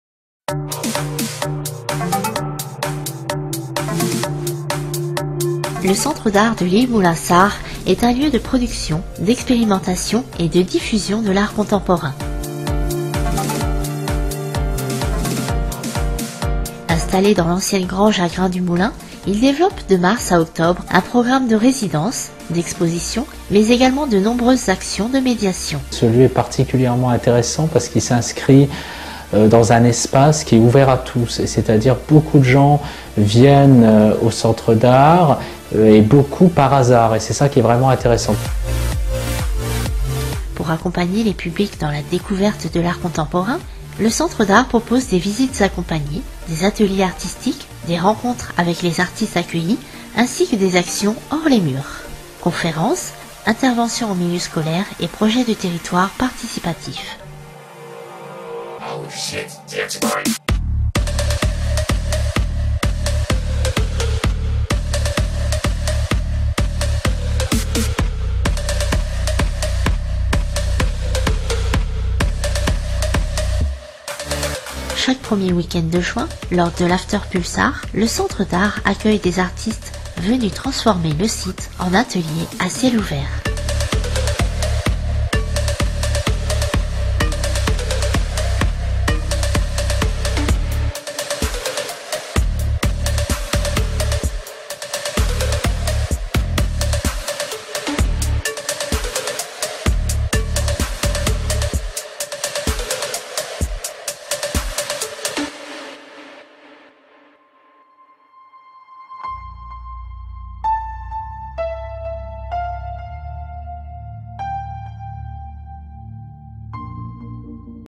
Découvrez le Centre d'art de l'île MoulinSart
Le Centre d'art de l'île MoulinSart est un lieu de production, d'expérimentation et de diffusion de l'art contemporain.

Situé dans l'ancienne grange à grains du moulin, il développe, de mars à octobre, un programme de résidences, d'expositions et également de nombreuses actions de médiation.

Pour accompagner les publics dans la découverte de l'art contemporain, le Centre d'art propose des visites guidées, des ateliers artistiques, des rencontres avec les artistes accueillis ainsi que des actions hors les murs : conférences, interventions en milieu scolaire et projets de territoire participatifs.